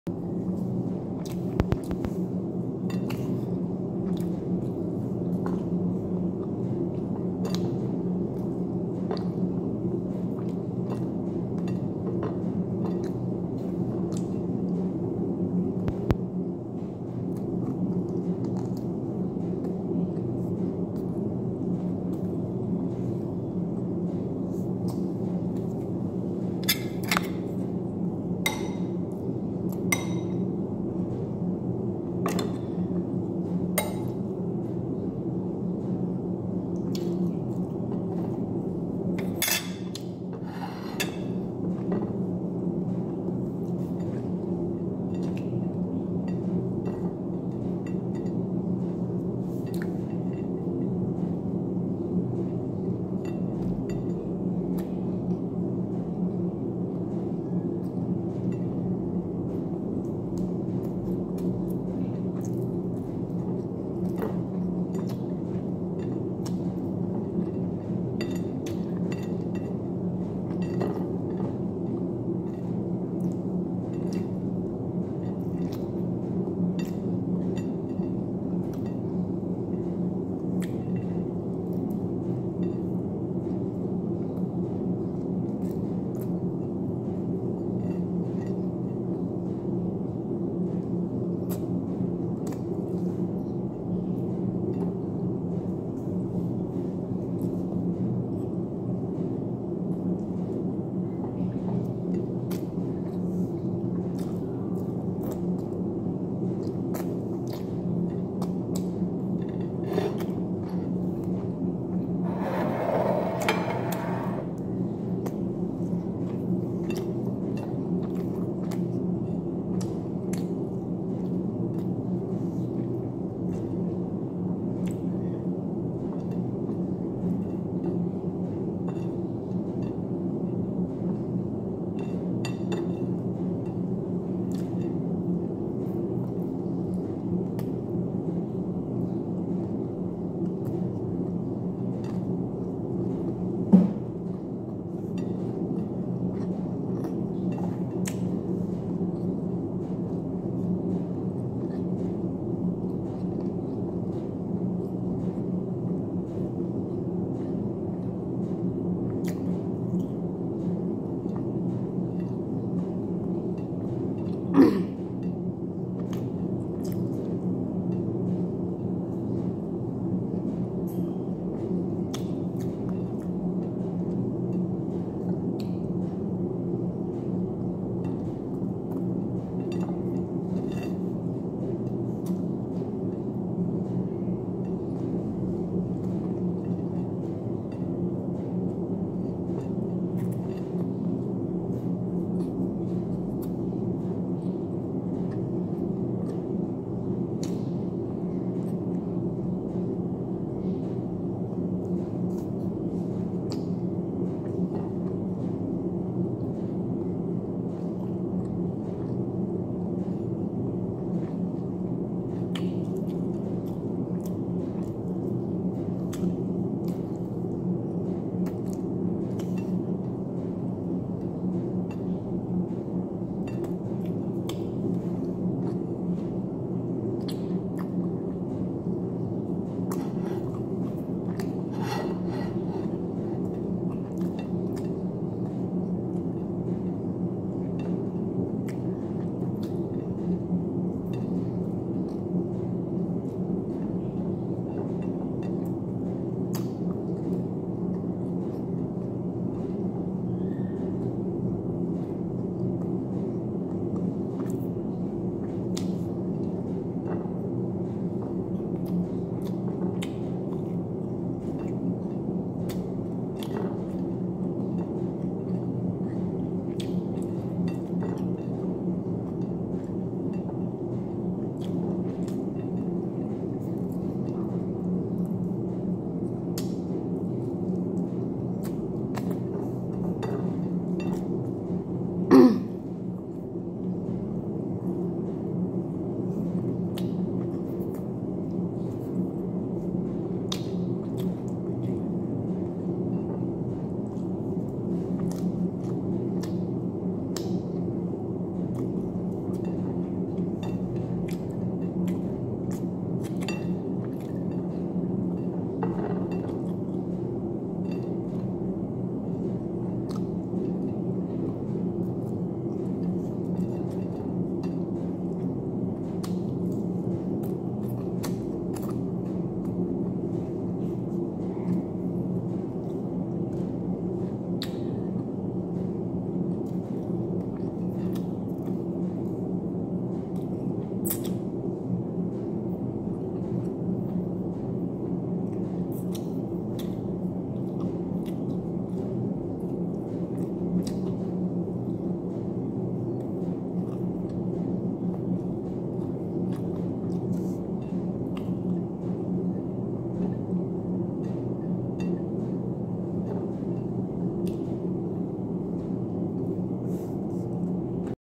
fried chicken wizard with white rice
fried chicken wizard cook with olive oil with chili power